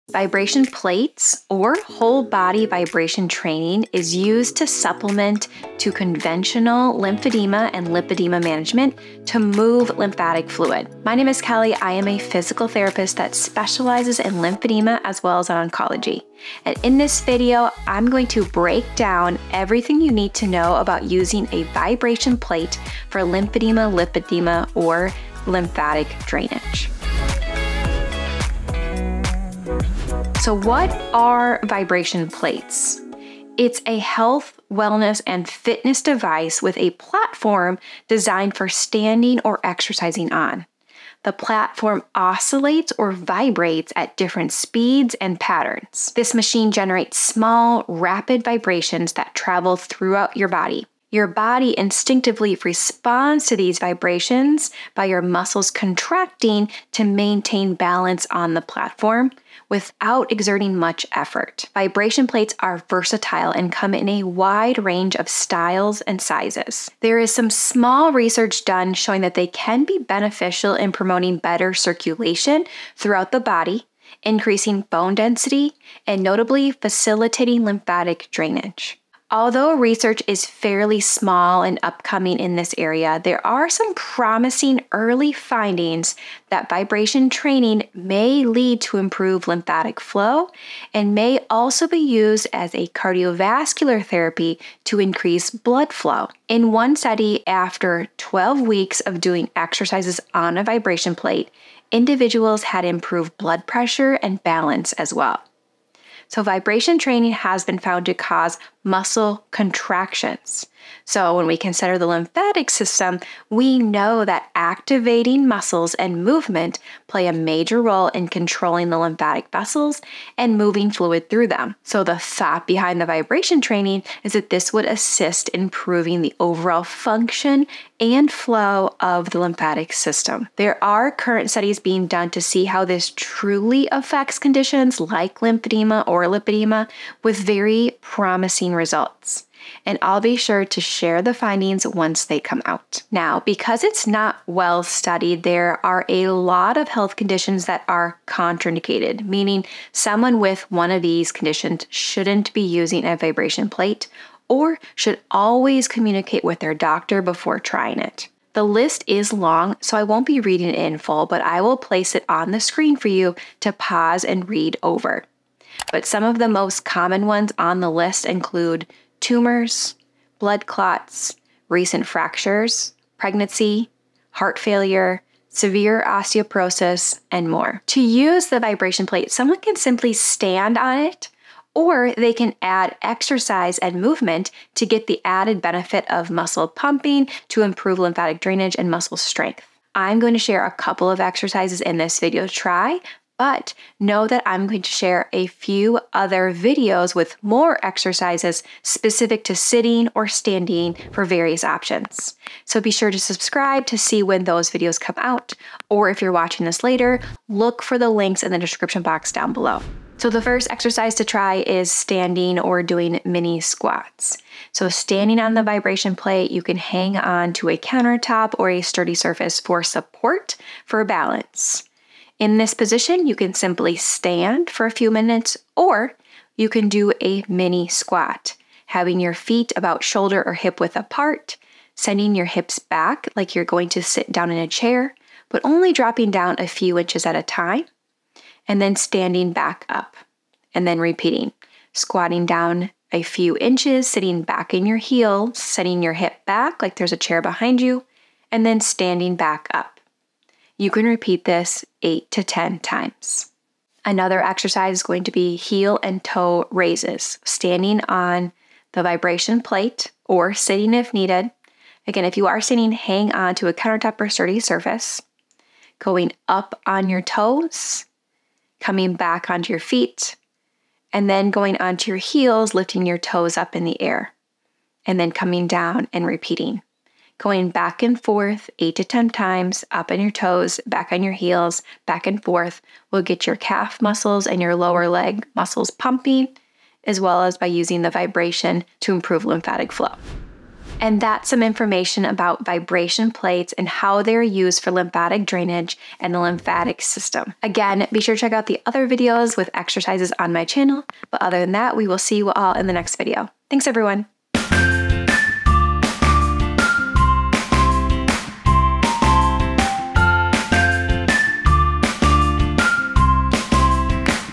Do Vibration Plates Work for Lymphatic Drainage?
Curious about using vibration plates for lymphatic drainage? You aren't alone! Vibration plates and use for lymphedema, lipedema, and the lymphatic system is the most common question I get asked about. Watch this video to find out if vibration plates really work for lymphatic drainage and how they can benefit your health.

(or any of their products)

Research referred to:
National Library of Medicine. Effects of Multidirectional Vibrations Delivered in a Horizontal Position (Andullation®) on Blood Microcirculation in Laboratory Animals: A Preliminary Study.

Lohman EB 3rd, Petrofsky JS, Maloney-Hinds C, Betts-Schwab H, Thorpe D. The effect of whole body vibration on lower extremity skin blood flow in normal subjects. Med Sci Monit. 2007;13(2):CR71-CR76.

Park SY, Son WM, Kwon OS. Effects of whole body vibration training on body composition, skeletal muscle strength, and cardiovascular health. J Exerc Rehabil. 2015 Dec 31;11(6):289-95. doi: 10.12965/jer.150254. PMID: 26730378; PMCID: PMC4697776.

* Although I do monitor comments, I cannot respond to personal medical questions. Please know that these are questions for your own doctor or provider as this is general information only. Thanks!